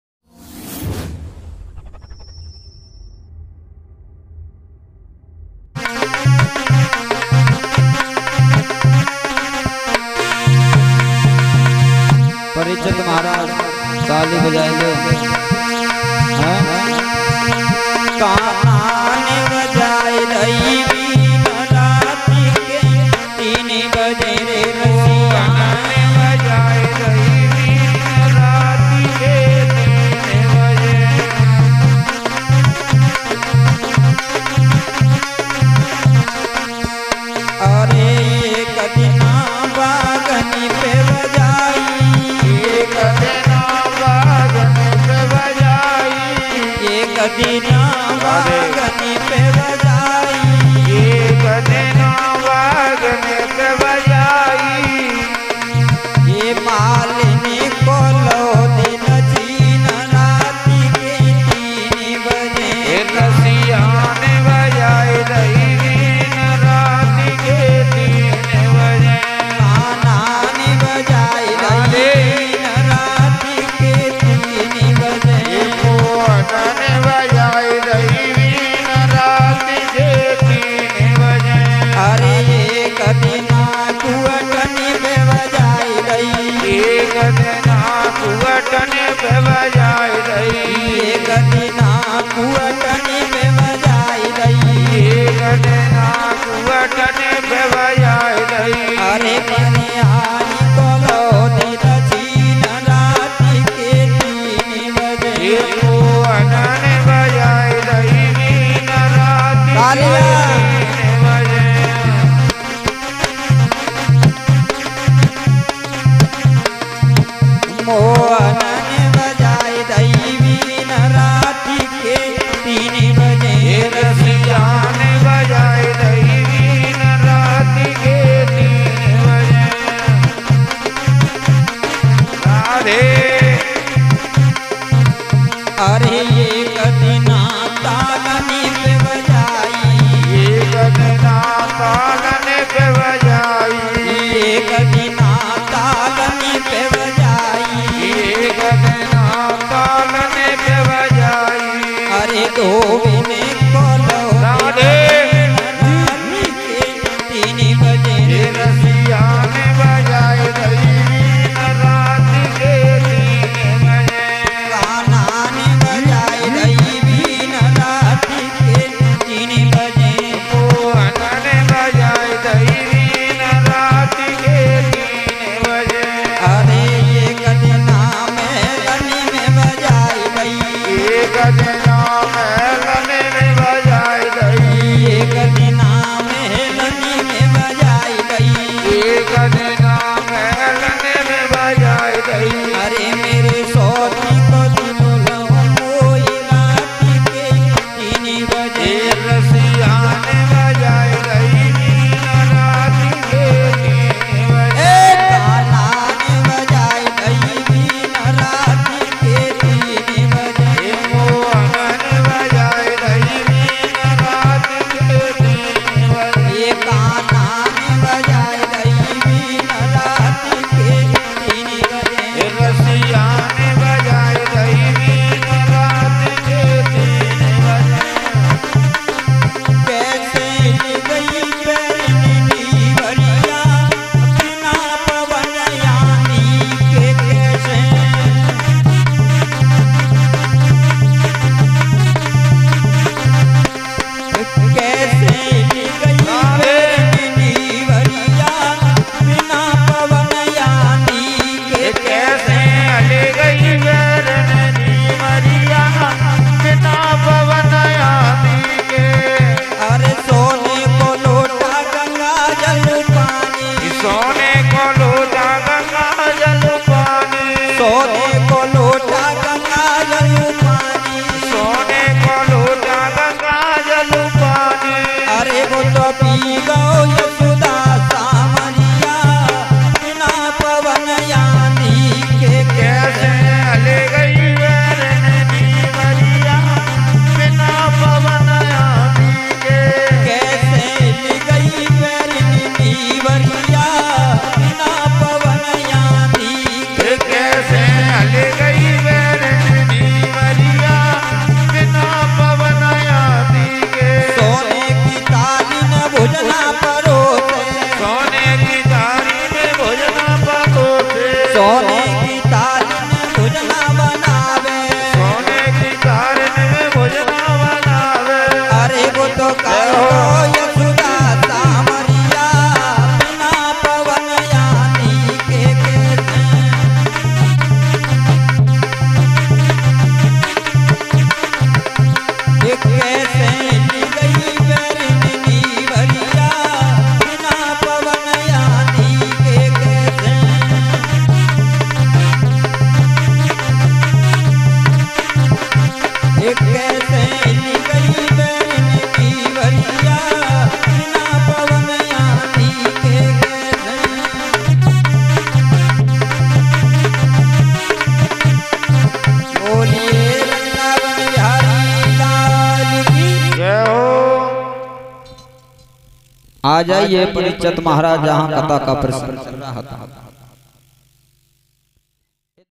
दोनों भाभियों ने बीच पंडाल में किया धांसू डांस गायक रामपुत्र शास्त्री ग्राम बाकलपुर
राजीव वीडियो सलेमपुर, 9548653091